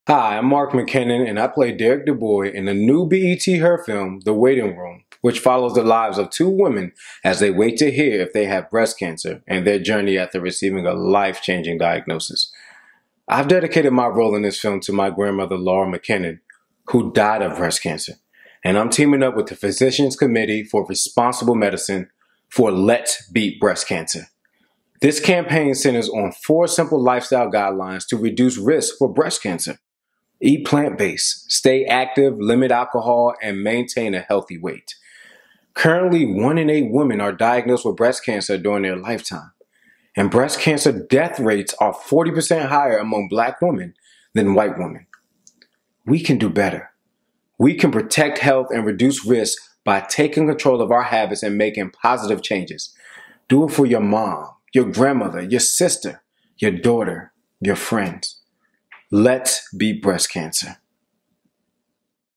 Let's Beat Breast Cancer | Mark McKinnon
Actor Mark McKinnon teams up with the Physicians Committee for Let's Beat Breast Cancer, a campaign to promote four healthy lifestyle guidelines to reduce risk for breast cancer or recurrence:

1) Choose plant-based foods
2) Exercise regularly
3) Limit alcohol
4) Maintain a healthy weight

Mark McKinnon stars in the new BET Her film “The Waiting Room” – which follows the lives of two women as they wait to hear if they have breast cancer and their journey after receiving the life-changing diagnosis. Mark has dedicated this role to both his grandmother who died of breast cancer and his father, who died of prostate cancer. The film was both directed and written by all Black women and is set for release on Oct. 17.